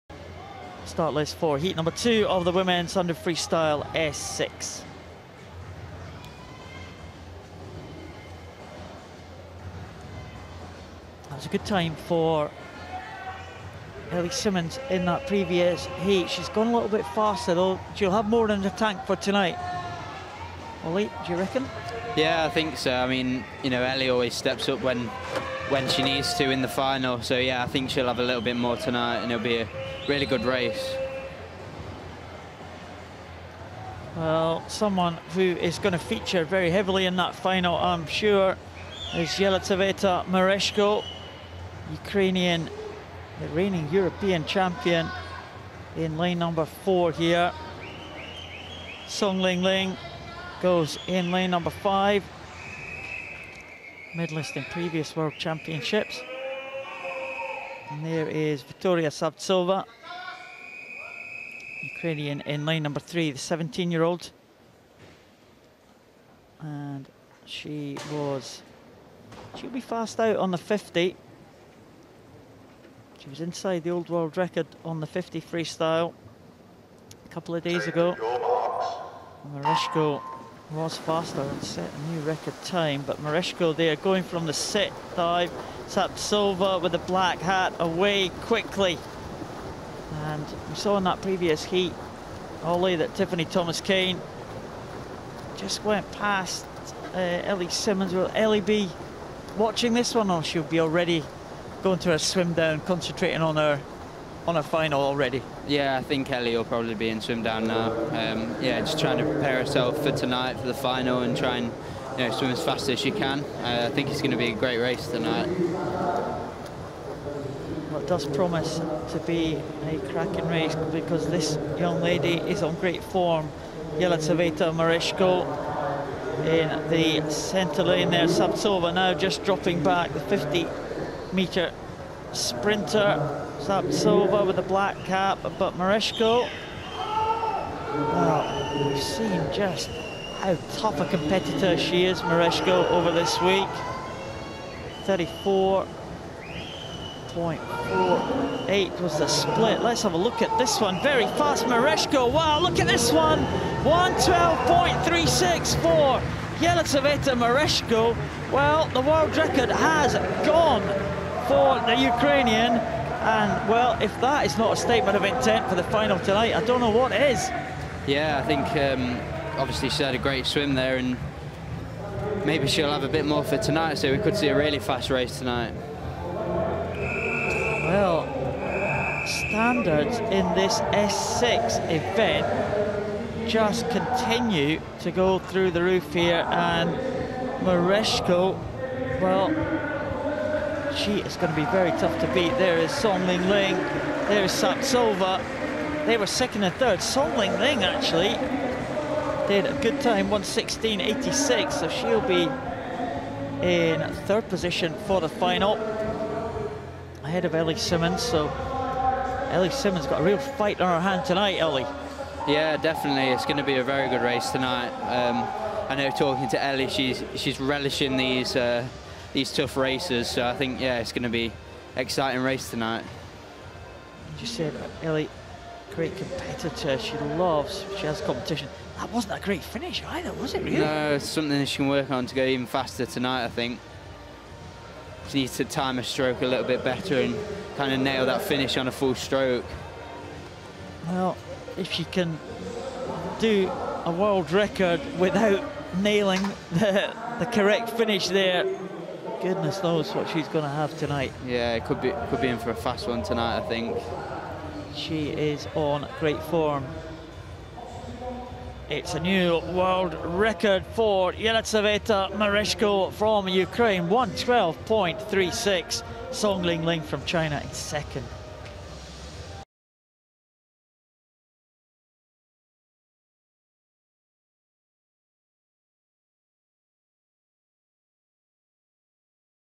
WORLD RECORD Women's 100m Freestyle S6 | Heat | 2015 IPC Swimming World Championships Glasgow
MERESHKO Yelyzaveta of Ukraine setting a new world's best in the women's 100m freestyle S6 in a time of 1:12.36.

Start list:

Glasgow, Great Britain hosted the IPC Swimming World Championships between 13-19 July 2015. Over 580 athletes from nearly 70 countries competed in the 2015 IPC Swimming World Championships, the seventh edition of the competition.

For more information on the IPC Swimming World Championships in Glasgow, please